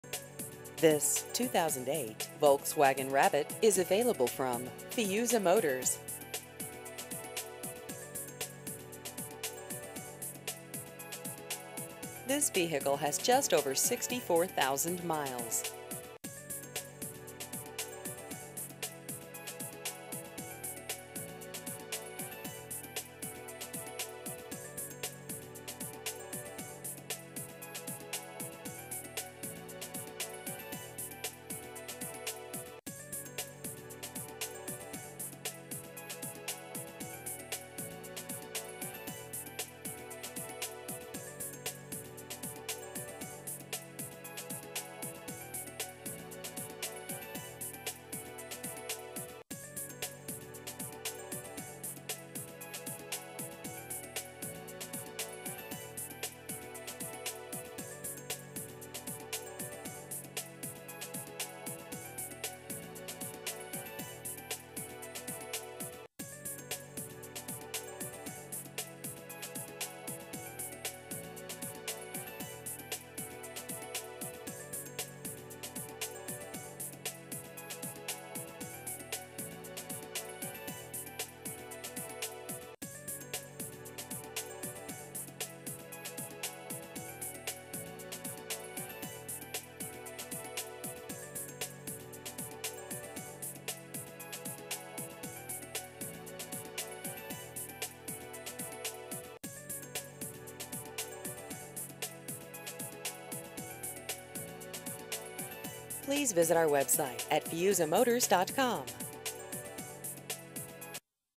2008 Volkswagen Rabbit Used Cars Utah Salt Lake City Midvale
This 2008 Volkswagen Rabbit is available from Fiuza Motors.